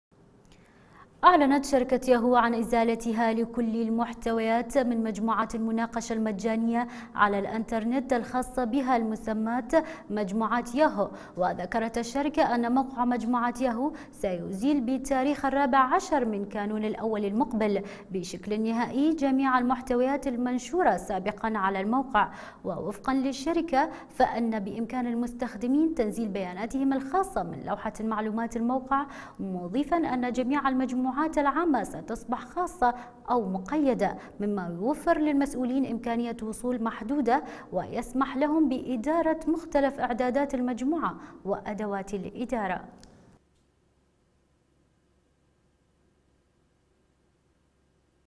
شركة ياهو ازالة المحتويات من مجموعات المناقشة المجانية على الإنترنت
شركة ياهو
ازالة المحتويات من مجموعات المناقشة المجانية على الإنترنت

أعلنت شركة ياهو عن إزالتها لكل المحتويات من مجموعات المناقشة المجانية على الإنترنت الخاصة بها المسماة مجموعات ياهو , وذكرت الشركة إن موقع مجموعات ياهو سيزيل بتاريخ الرابع عشر من كانون الاول المقبل  بشكل نهائي جميع المحتويات المنشورة سابقًا على الموقع، ووفقًا للشركة، فإن بإمكان المستخدمين تنزيل بياناتهم الخاصة من لوحة معلومات الموقع، مضيفة أن جميع المجموعات العامة ستصبح خاصة أو مقيدة، مما يوفر للمسؤولين إمكانية وصول محدودة ويسمح لهم بإدارة مختلف إعدادات المجموعة وأدوات الإدارة